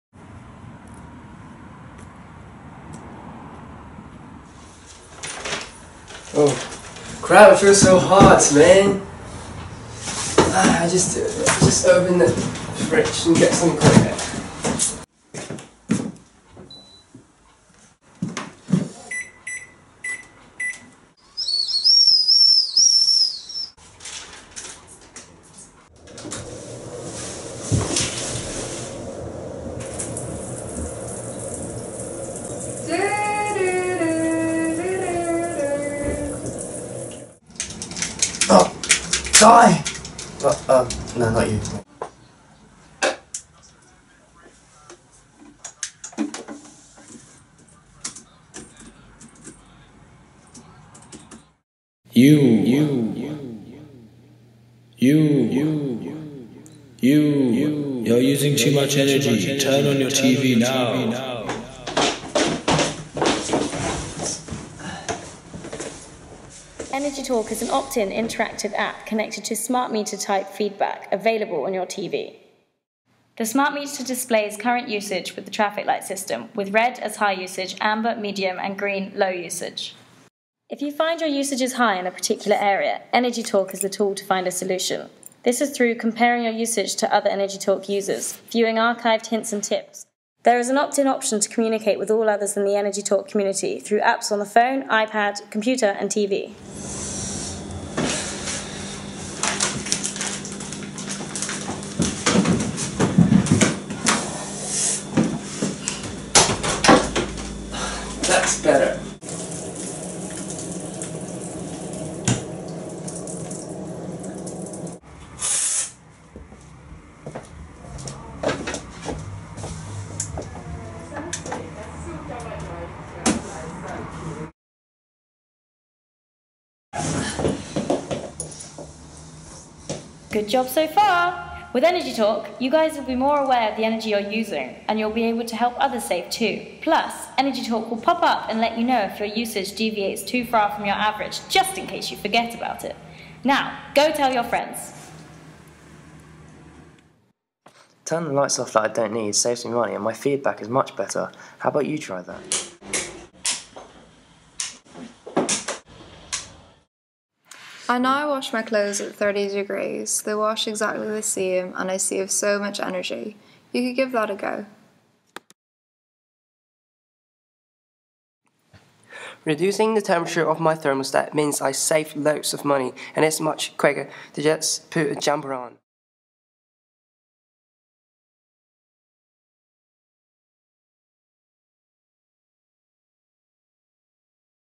Energy Talk
A short film made by students on the Energy and People module in the School of Environmental Sciences, University of East Anglia (March 2014).